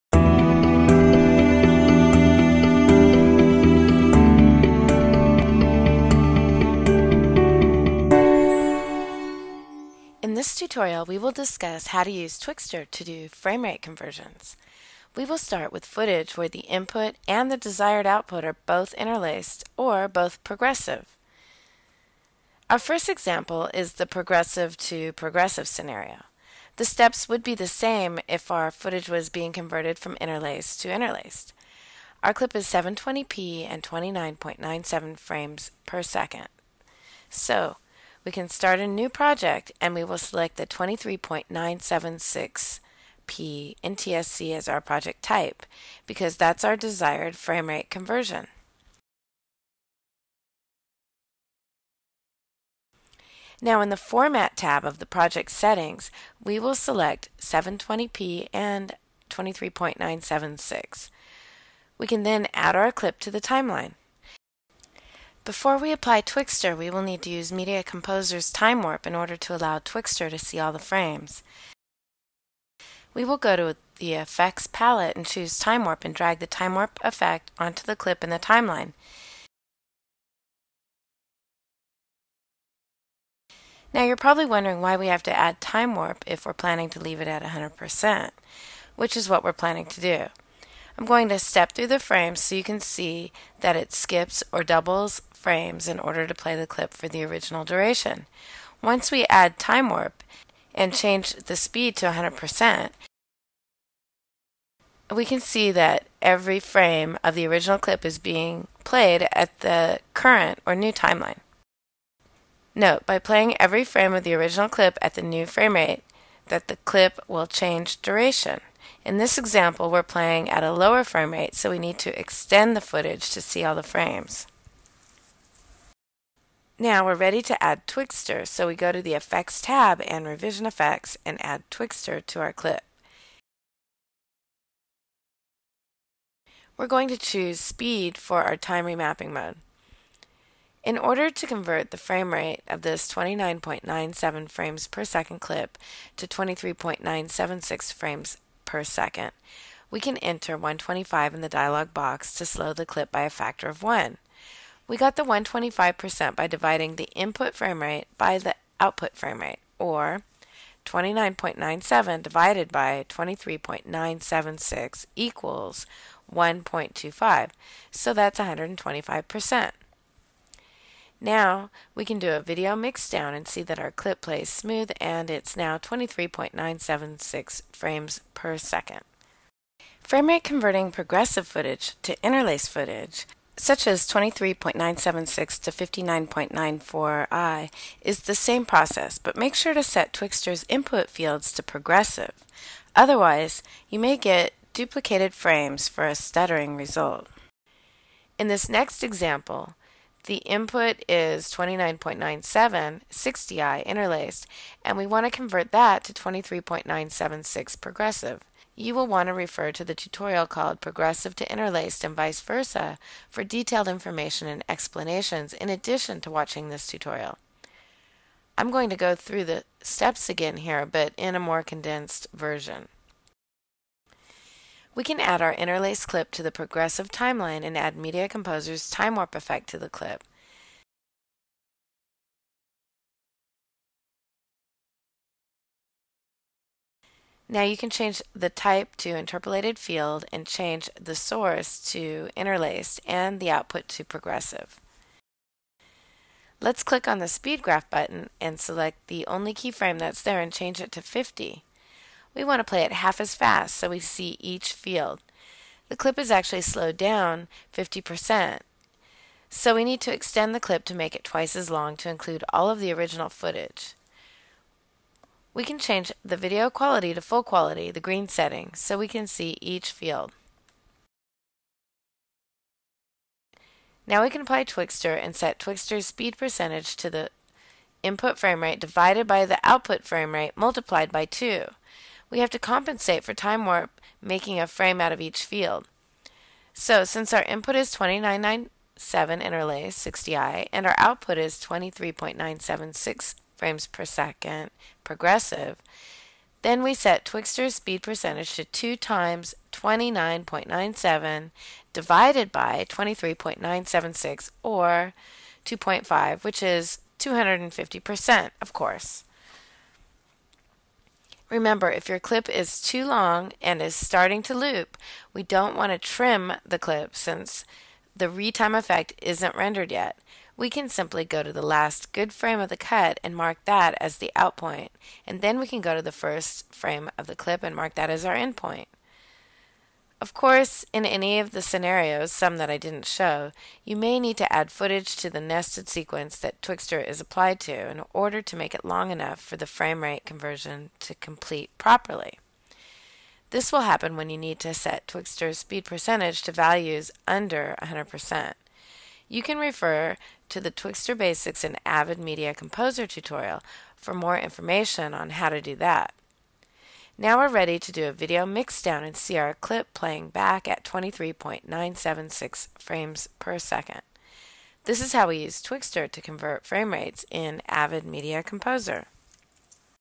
Twixtor in Avid MC: Frame Rate Conversions
This tutorial shows how how to do a Frame rate conversion if the Input and the Output are both Interlaced or both Progressive and also how to do a Frame Rate Conversion from Progressive Footage to Interlaced Footage in  Avid Media Composer

Special Thanks to Mark Tekushan for allowing us to use his sailing footage and also special thanks to Rickard Bengtsson for allowing us to use his water balloon footage